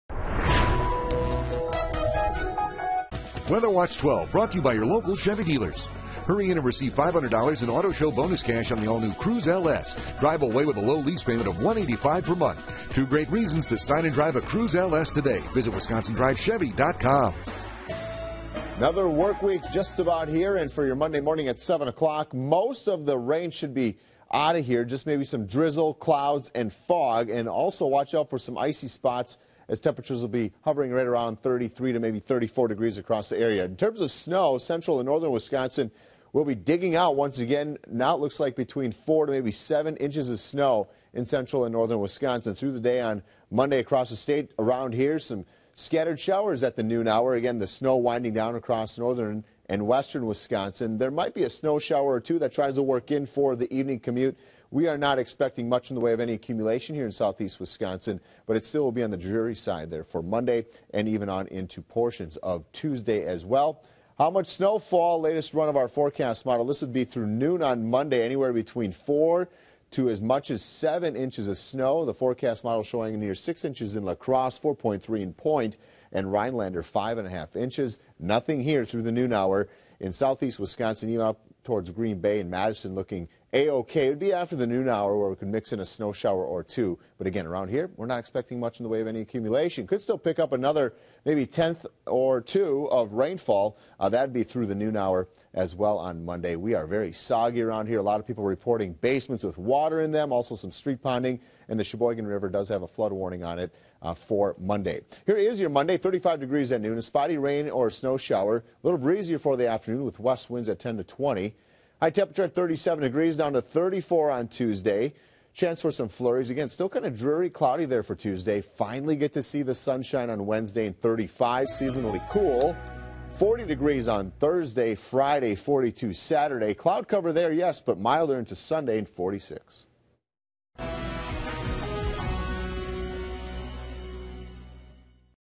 Luke Sampe's Forecast
Here is your Weather Watch 12 forecast from Meteorologist Luke Sampe.